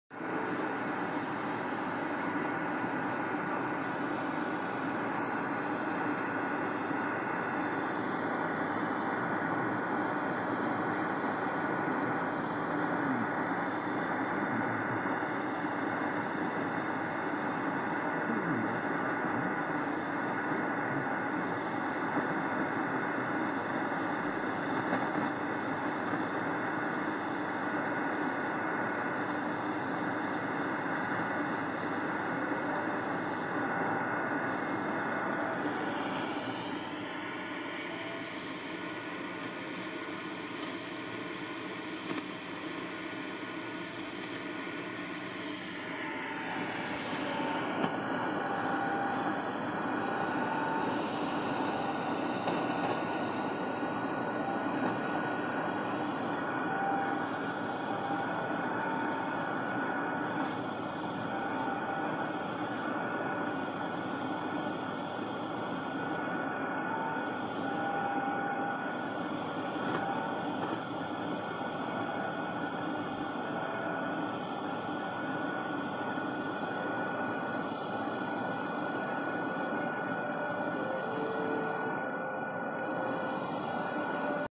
5980 kHz R.Chaski Red Integridad - Peru. Barely audible above the noise limit.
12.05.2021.
20.54 UTC
Rx Location: Hungary
Distance/Power: 11037km / ?